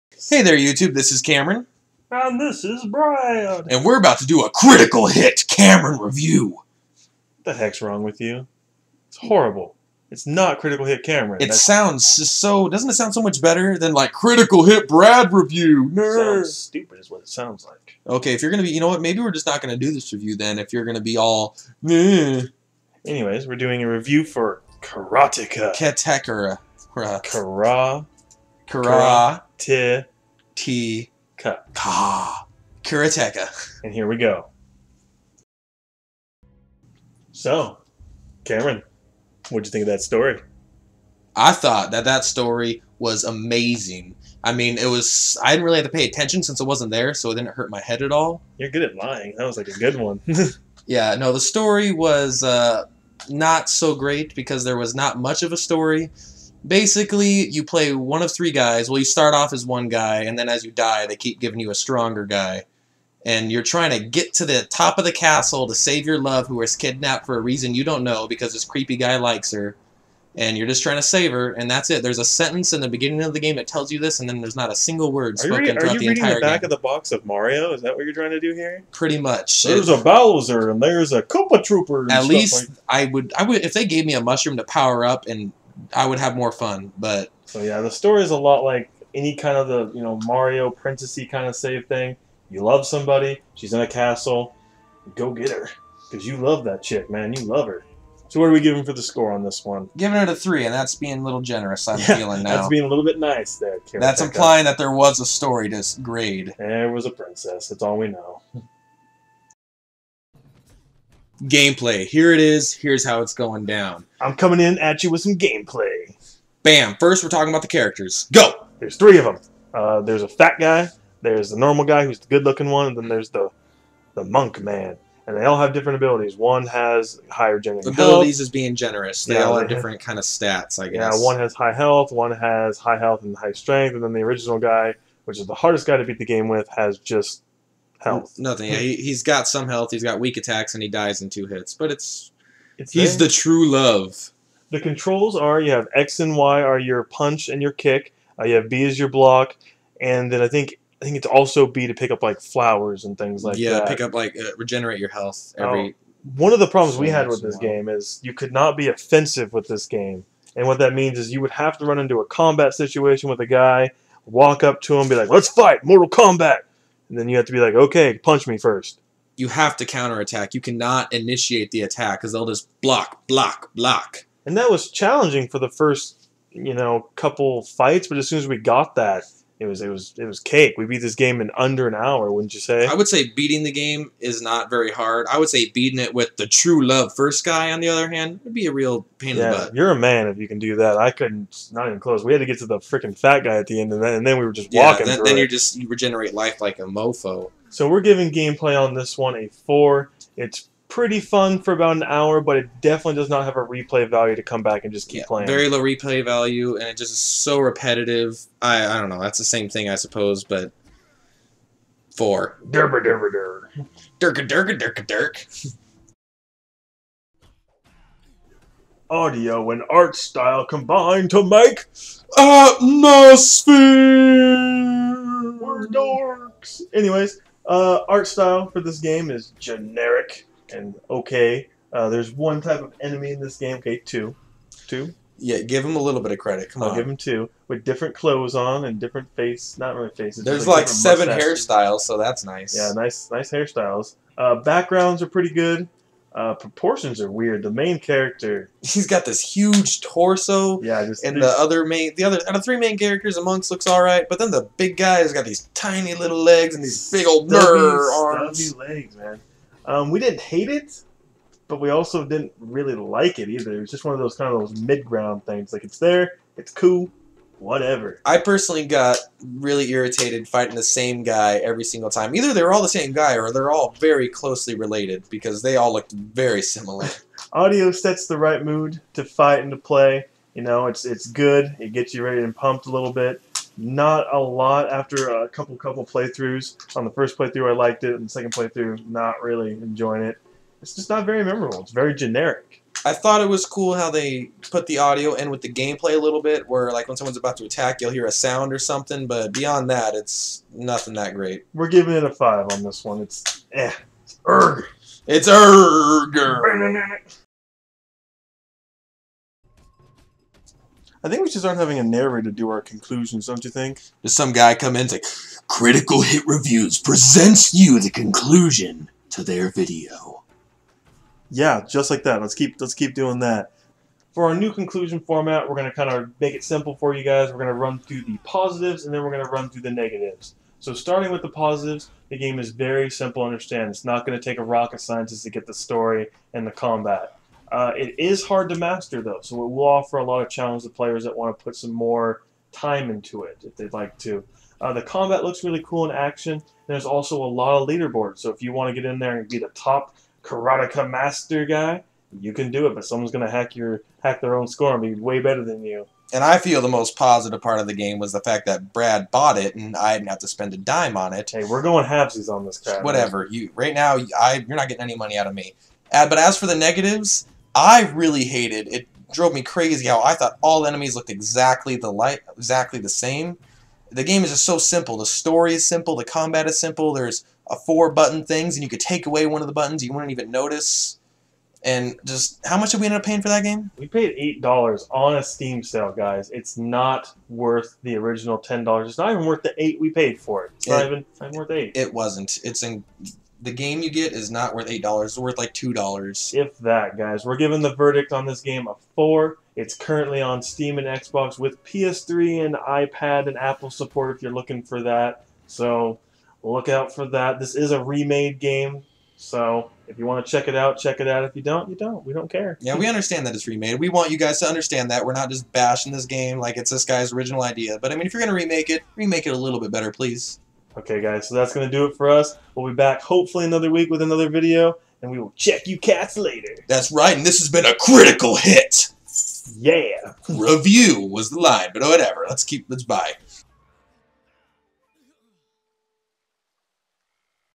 Critical Hit Review: Karateka
Critical Hit Review does a review of the remake of the 1984 game Karateka currently on XBLA and Steam. Do we like it? Well watch our review and find out.